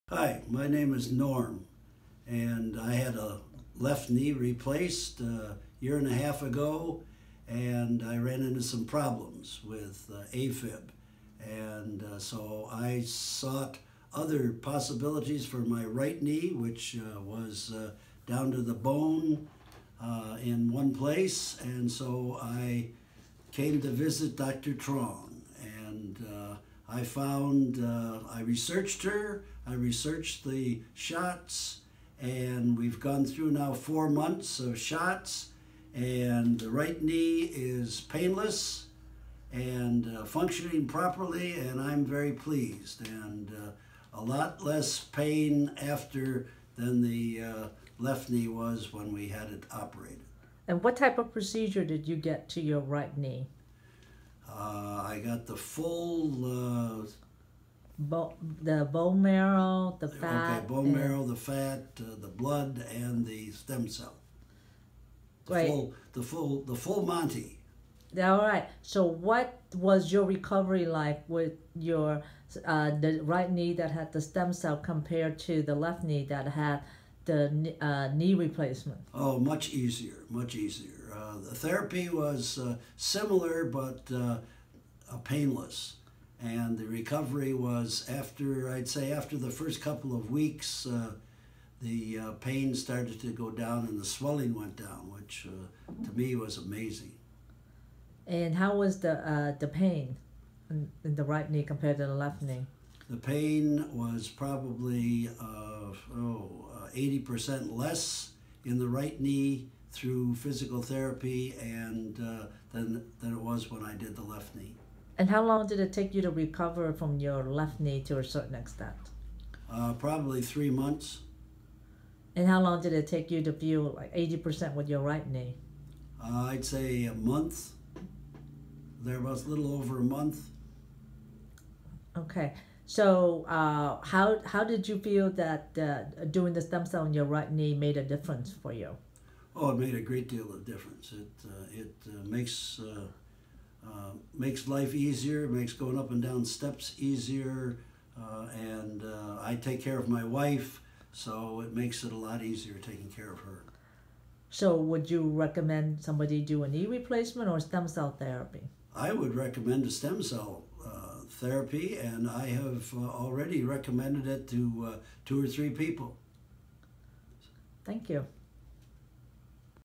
Norman Stem Cell Treatment for Knee
Comparison of Stem Cell Treatment vs Knee Replacement Surgery in a patient who had both.  Superior result from stem cell treatment with bone marrow, fat graft, and PRP.